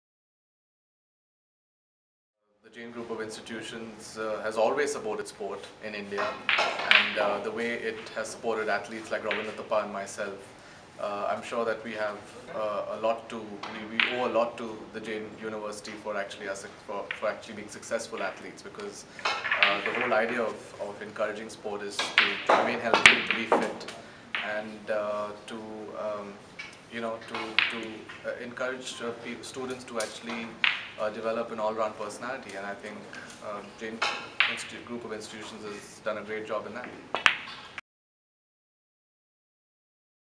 Celebrity Speak: Pankaj Advani
Pankaj Advani, World Billiards & Sporting Champion is the alumnus of Jain University. Tune into this video to view Pankaj Advani talking about the Jain University’s Sporting Culture!